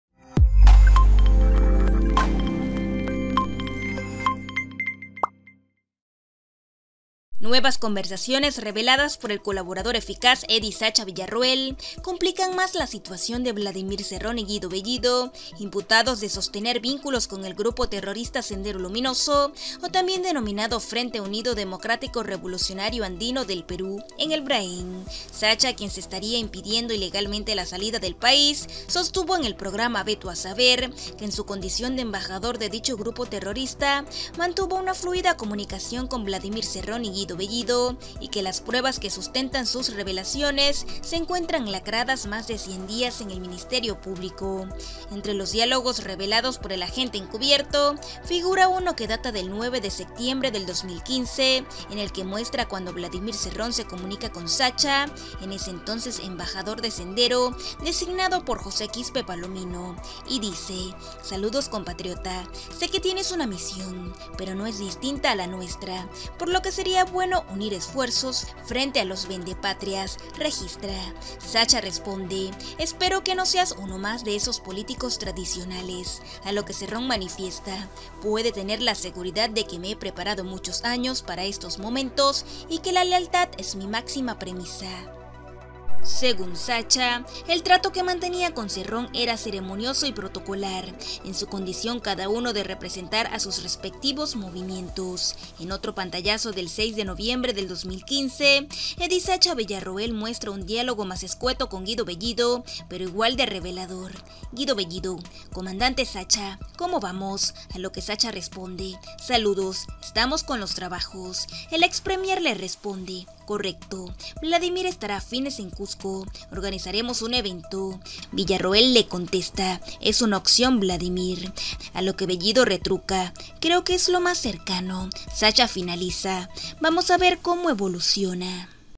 NUEVOS 'CHATS' HUNDEN MÁS A VLADIMIR CERRÓN Y GUIDO BELLIDO: EDDY "SACHA" VILLAROEL DINFUDIÓ PRUEBAS
Nuevas conversaciones reveladas por el colaborador eficaz Eddy “Sacha” Villarroel, complican más la situación de Vladimir Cerrón y Guido Bellido, imputados de sostener vínculos con el grupo terrorista Sendero Luminoso o también denominado Frente Unido Democrático Revolucionario Andino del Perú, en el Vraem.